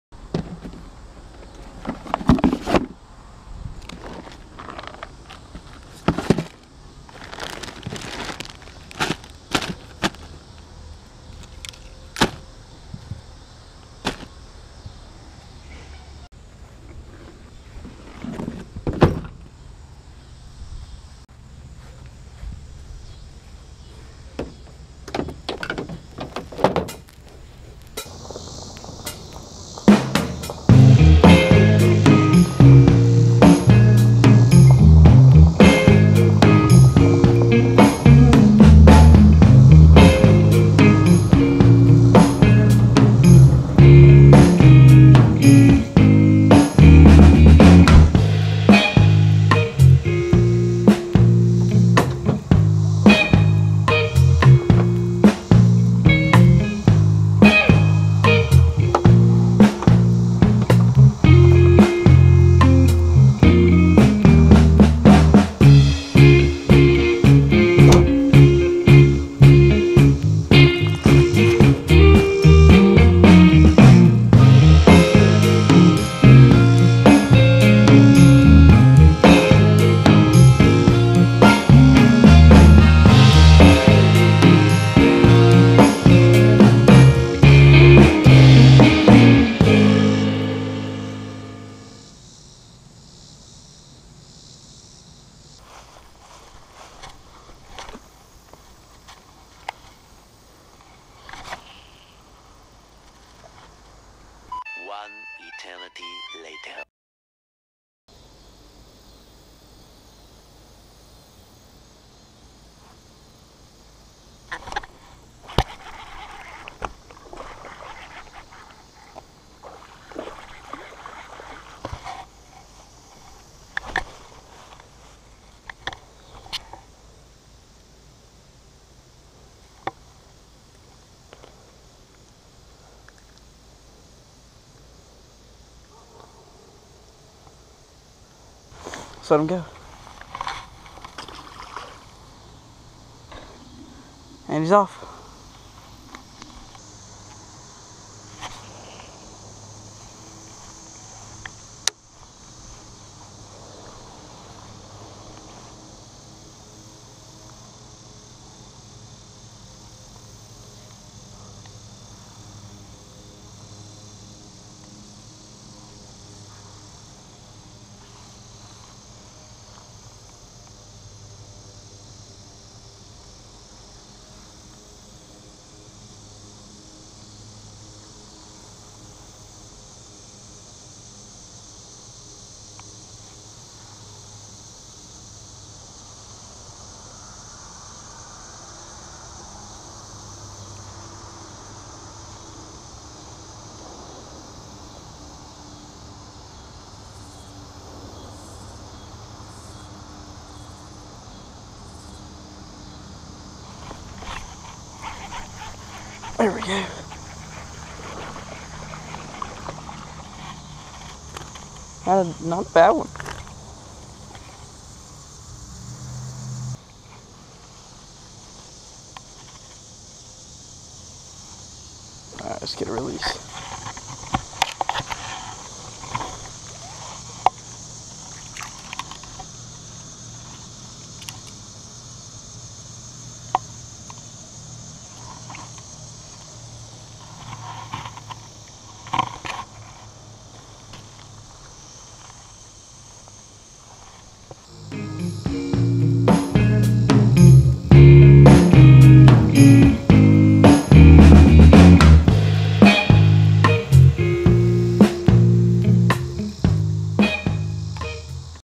Just another day
Just another day at the pond doing what I do almost the best !

Lure
Strike King Rage Craw Green Pumpkin
Berkley Powerbait 7" blue fleck ribbon tail worm
3/0 Gamagatsu EWG
1/8 Strike King Tungsten

Line
BPS Excel 10 Pound Mono

Reel
Lews LFS SpeedSpool 7:5.1

Rod
7'1 MH 13 Fishing Omen Black Fast

Camera
GoPro Hero 3+ with Chesty Mount and homemade Tripod